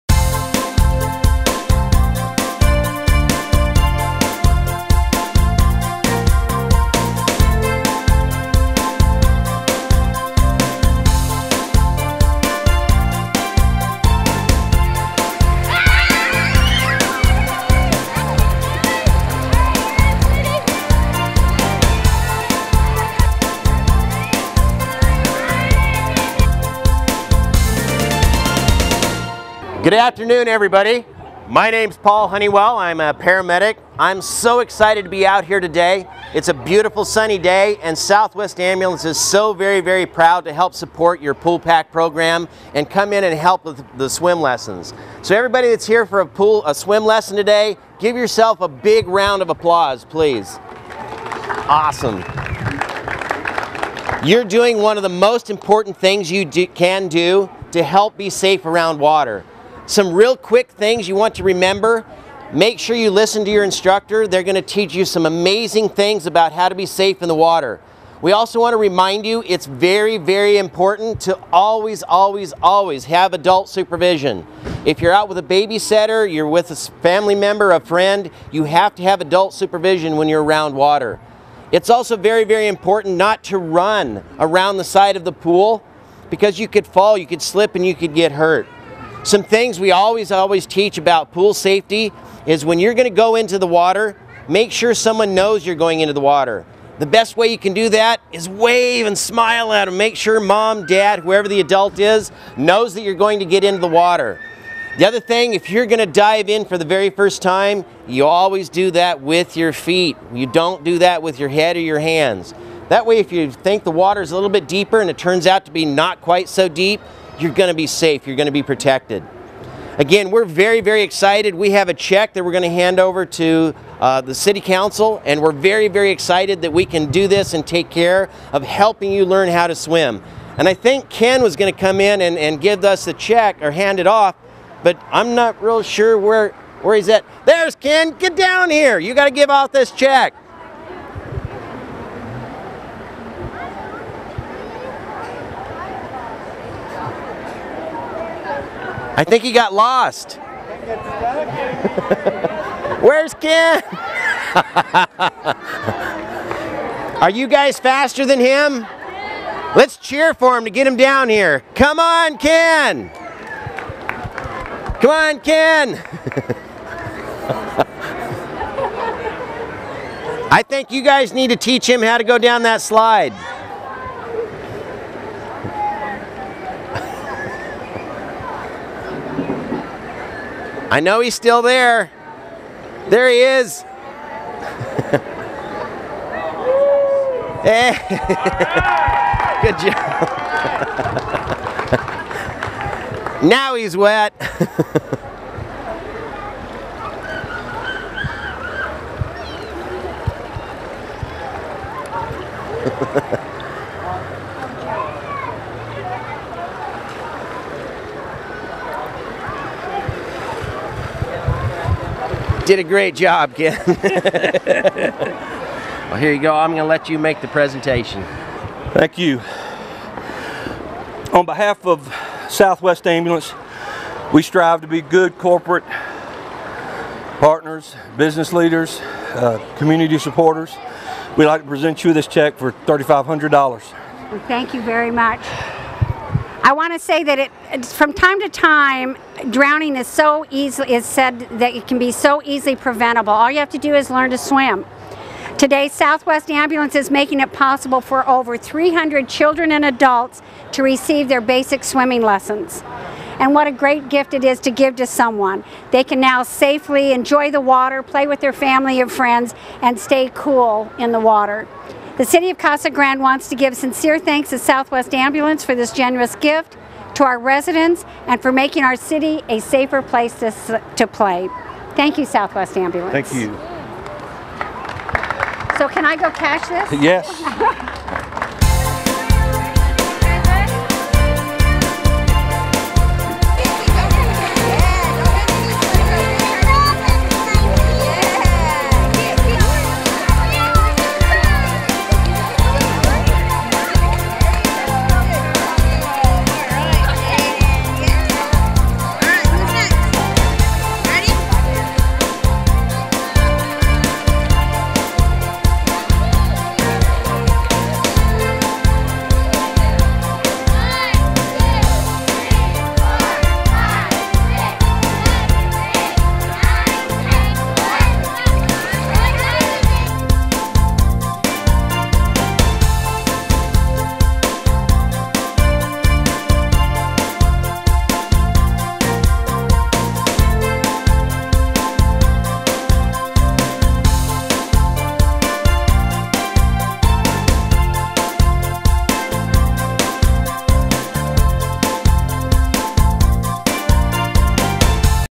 Southwest Ambulance Pool Pack Program
Southwest Ambulance presented a $3500 check to the City of Casa Grande as part of their Pool Pack Program to reduce the number of drownings.  Free swim lessons are available at the Palm Island Family Aquatic Park.